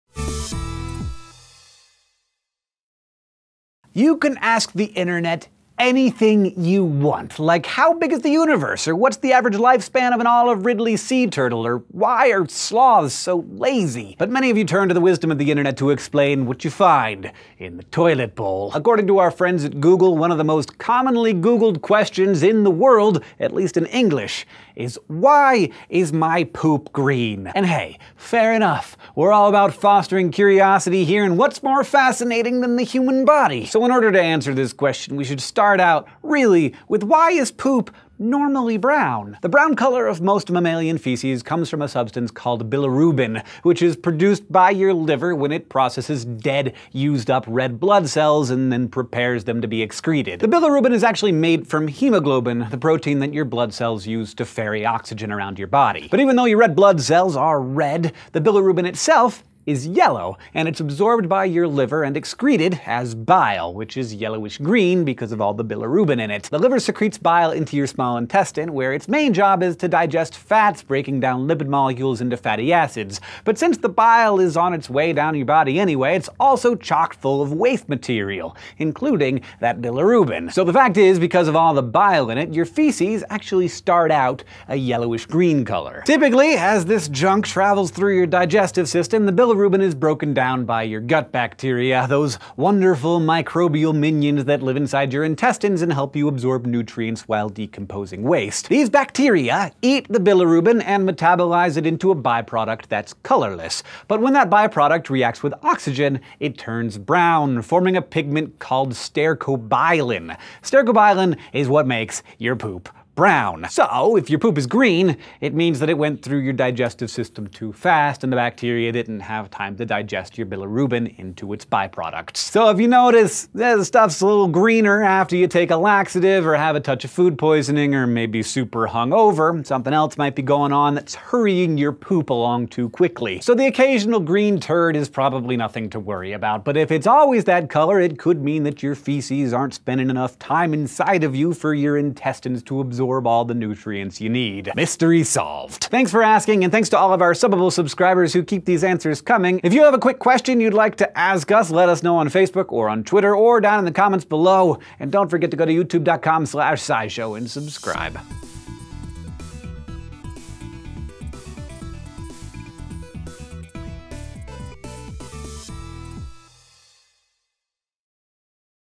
Why Is My Poop Green?
One of the most commonly googled questions in the world is why feces can be green. Well, Quick Questions has the answer!